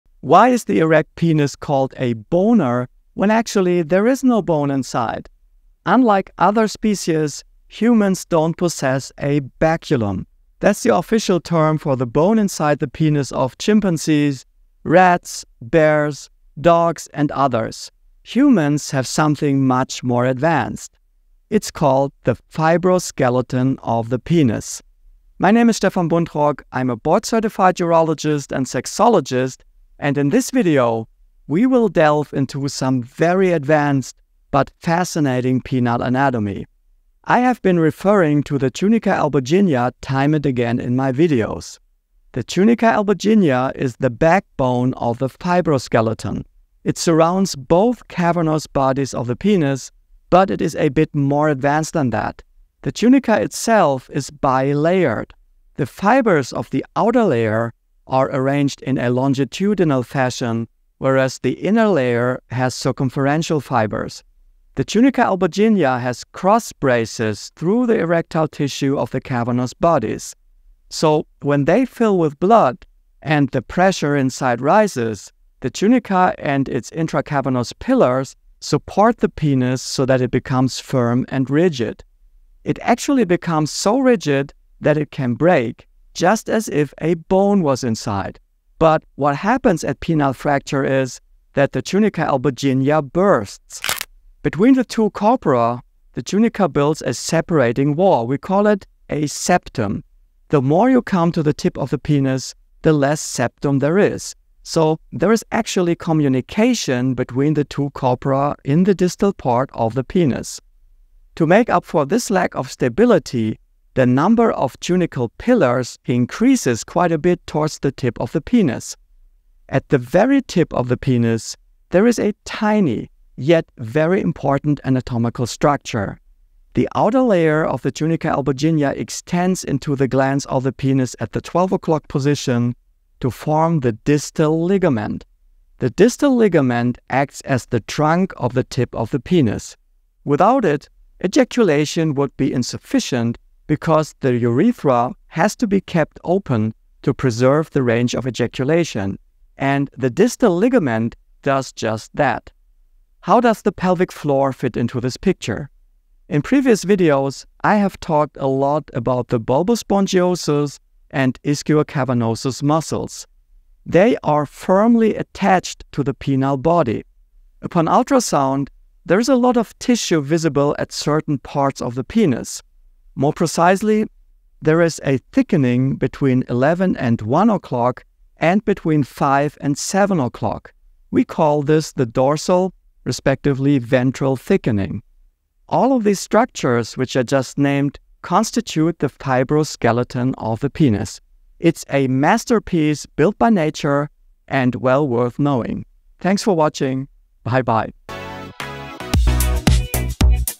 Human penis - no bones - but why so rigid? | UroChannel
Why is an erect penis playfully dubbed a "boner" when, in reality, there isn't a single bone to be found? While other species are equipped with a penis bone, known as the baculum, humans possess something far more intricate and advanced – the fibro-skeleton of the penis.
Meet Dr. med. Dr. phil. Stefan Buntrock, a board-certified urologist and sexologist from Göttingen, here to unravel the captivating secrets of penile anatomy. In his video, he takes us on a journey into the fascinating world of the penile fibro-skeleton.
At the heart of this marvel is the tunica albuginea, a bi-layered structure that envelops the cavernous bodies of the penis. The outer layer's longitudinal fibers and the inner layer's circumferential fibers create a robust framework that is crucial for the rigidity of an erect penis. When engorged with blood, this structure, akin to a bone, lends unwavering support. Yet, it's not entirely indestructible, as a penile fracture can rupture the tunica albuginea.
But that's not all; hidden within this intricate system is the distal ligament, a key player in maintaining ejaculatory functionality. Beyond the anatomical wonders, the pelvic floor, bulbospongiosus, and ischiocavernousus muscles come into play, providing further insight into the fibro-skeleton's complexity.